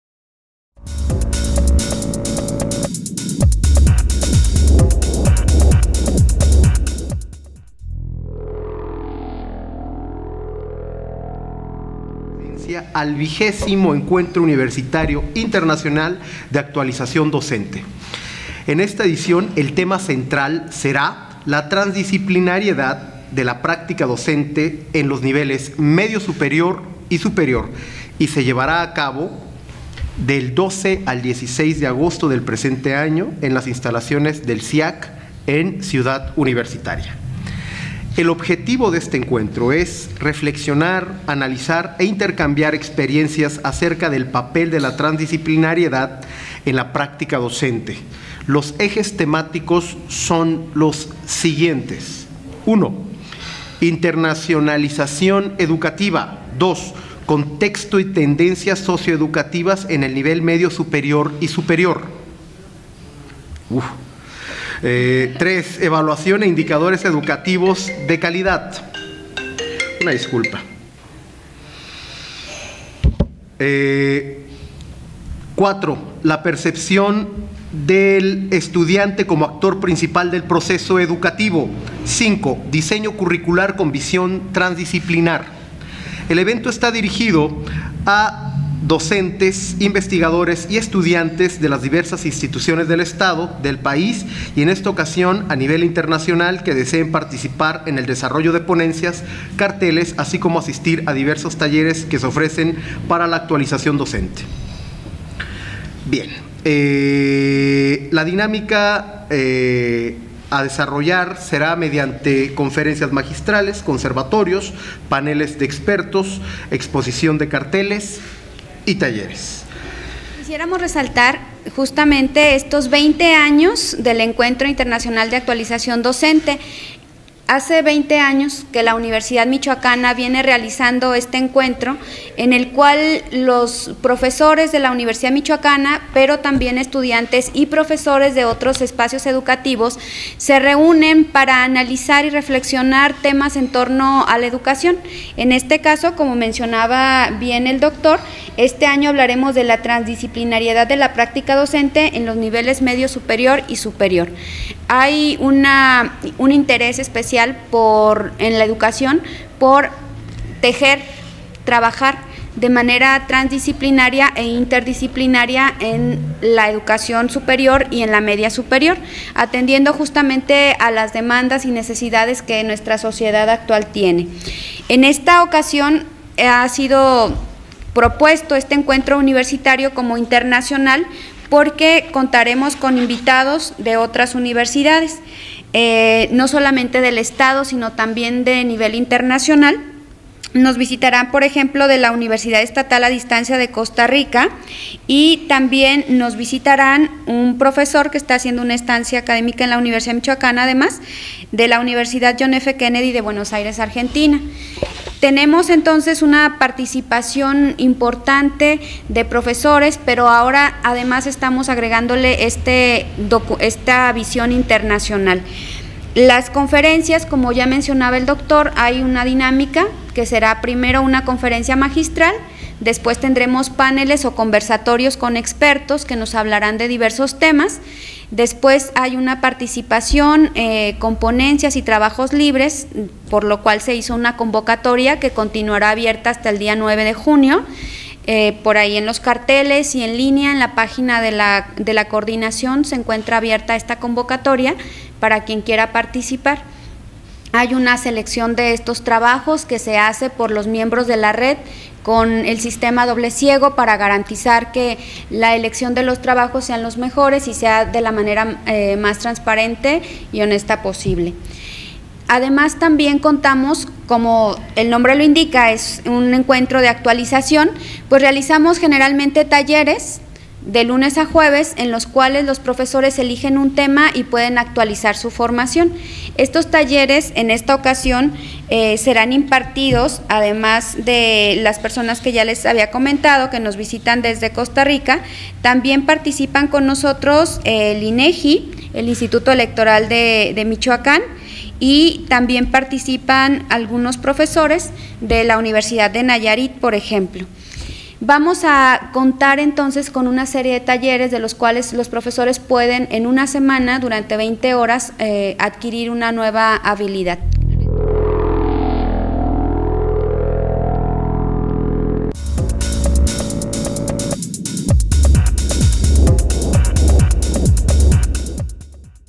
XX ENCUENTRO DE ACTUALIZACIÓN DOCENTE , RUEDA DE PRENSA
Con el objetivo de reflexionar, analizar e intercambiar experiencias acerca del papel de la transdisciplinariedad en la práctica docente, la Universidad Michoacana de San Nicolás de Hidalgo será la sede durante el próximo mes de agosto del XX Encuentro Universitario Internacional de Actualización Docente.

El evento está dirigido a docentes e investigadores de las diversas instituciones del estado y del país que deseen participar con el desarrollo de ponencias de investigación bajo los siguientes ejes temáticos: Internacionalización educativa; Contexto y tendencias socioeducativas en el nivel medio superior y superior; Evaluación e indicadores educativos de calidad; La percepción del estudiante como actor principal del proceso educativo; y Diseño curricular con visión transdisciplinar.

Se llevará a cabo del 12 al 19 de agosto del presente año y cuenta con un programa que incluye una serie de ponencias, carteles, conferencias, talleres, mesas de trabajo y paneles, que versarán sobre el tema principal del evento “Transdisciplinariedad de la práctica docente en los niveles Medio Superior y Superior”.

La fecha límite para la entrega de resúmenes de ponencias y talleres concluye este próximo 23 de mayo, en tanto que la notificación de los resultados deberá realizarse antes del 7 de junio.

Para más información dirigirse a la Coordinación de Responsabilidad Social y Formación Docente, en la planta alta del edificio “M” en Ciudad Universitaria, teléfono o bien consultar la convocatoria en la página